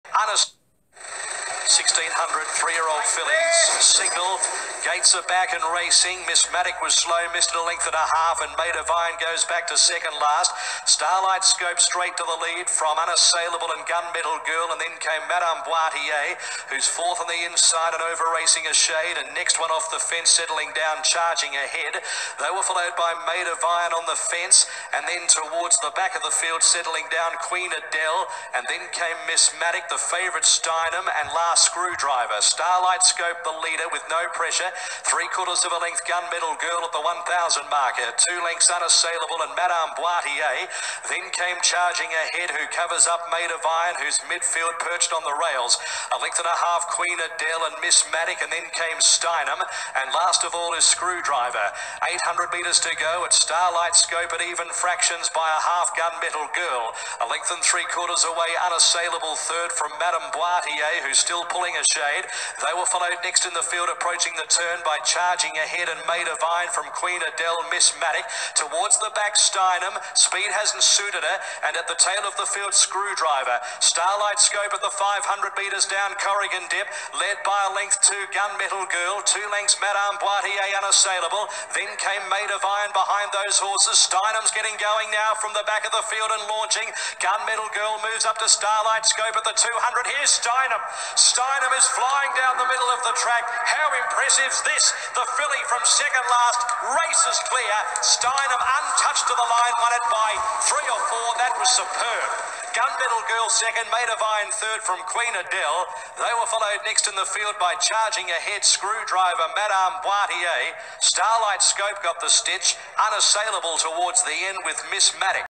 STEINEM (GB) Frankel - Thai Noon filly, Sandown R3 31/03/2021 - 3-Y-O Fillies BM64 Hcp
1600m Good4 Out 7m Entire Circuit $50K Jye McNeil (8) 59.5 (L: 56) Flucs: $0 $2.20 F; 1:37.120 2-Gunmetal Girl (Mark Zahra, 9) 60.5 , 3-Maid Of Iron (Lachlan King, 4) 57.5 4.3L , 4-Queen Adele (Kyle Maskiell, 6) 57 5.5L . Rtg: 63, Steinem is 2 Wins from 2 Starts